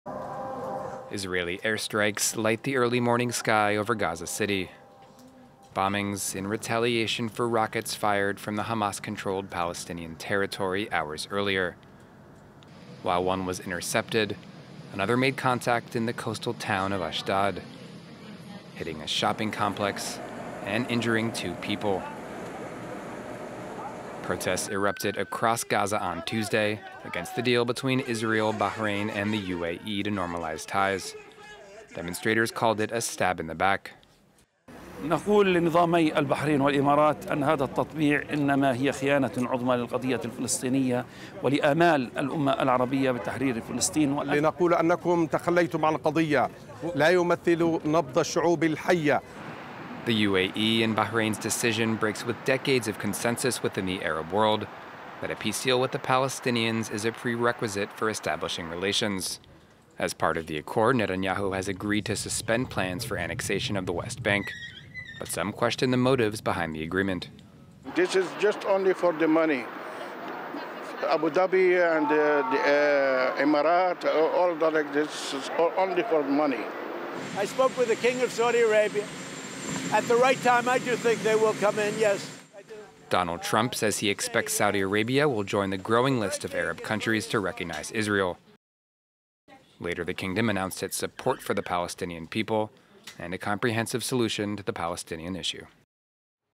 Israel strikes Gaza after rocket fire during US ceremony
The Israeli military struck Hamas militant sites in the Gaza Strip early Wednesday in response to rocket fire toward Israel the previous night that coincided with the signing of normalisation agreements between Israel and two Arab countries at the White House.